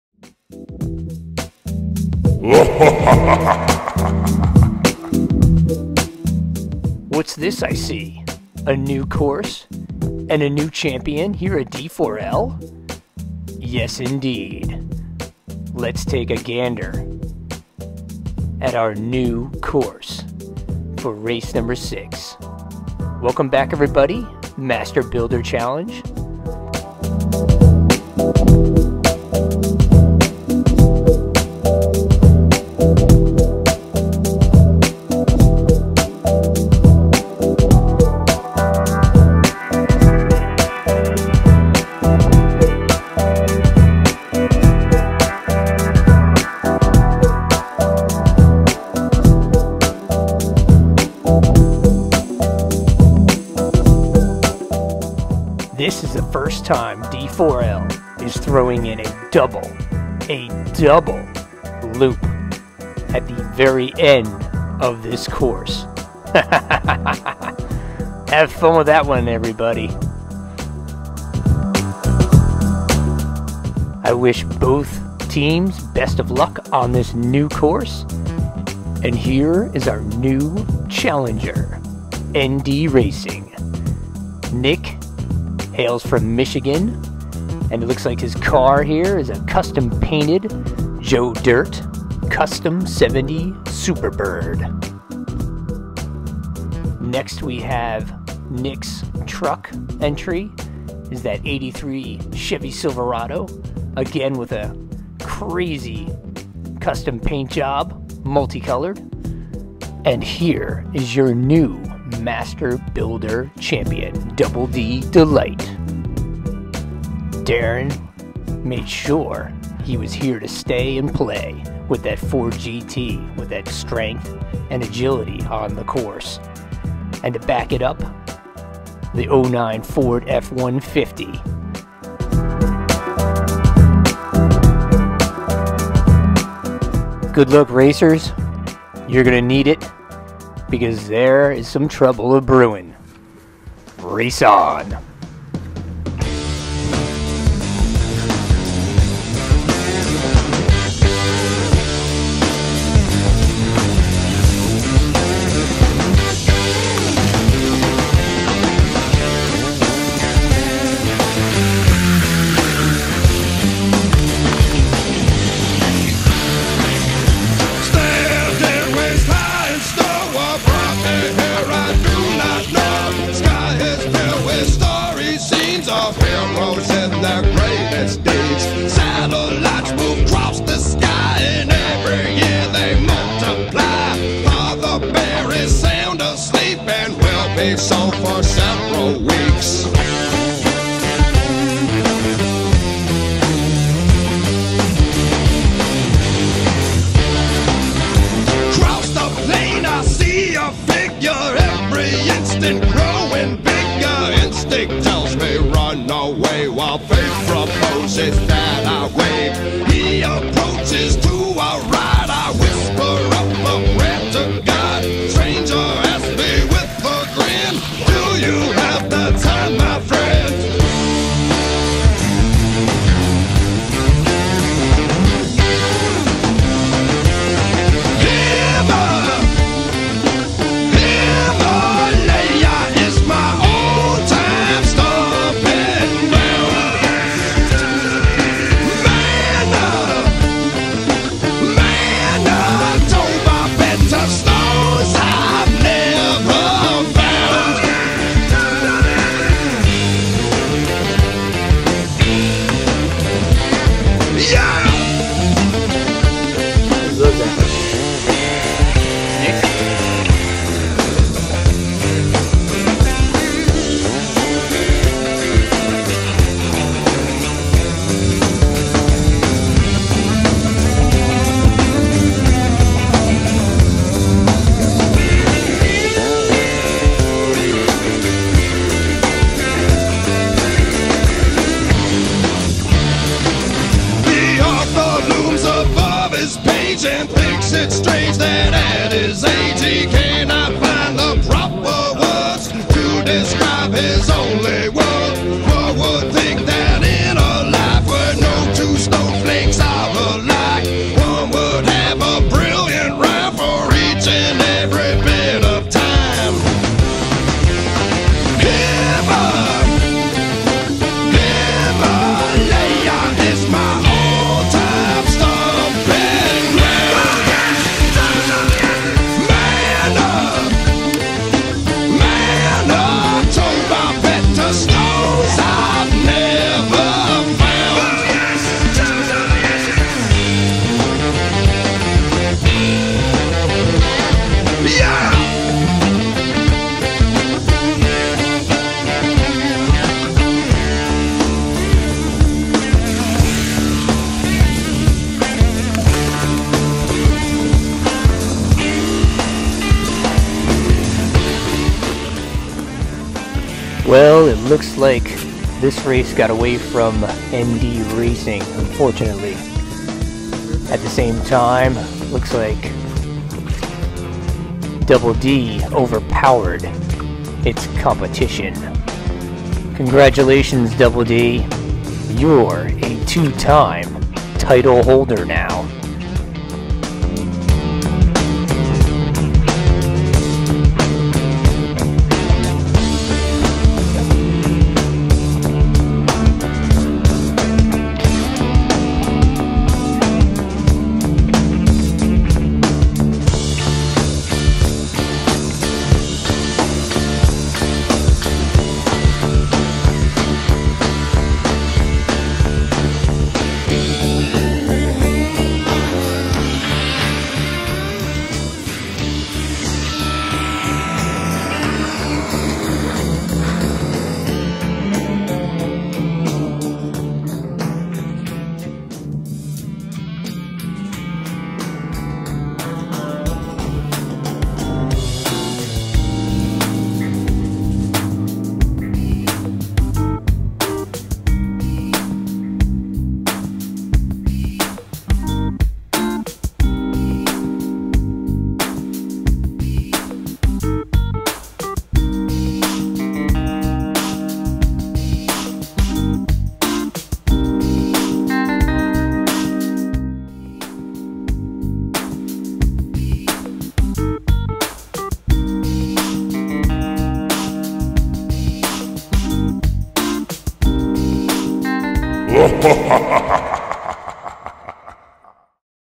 HOT WHEELS "SUPER MODIFIED" RACING -D4L-2019 MASTER BUILDER RACE06
Double "D" Delight now holds the Title and put on quite a performance against Faüs Racing to get it! Now, ND Racing wants a piece of action, and the Title to boot! Let's see if ND Racing can pull it off. Enjoy!
Race-On! & Race-Hard!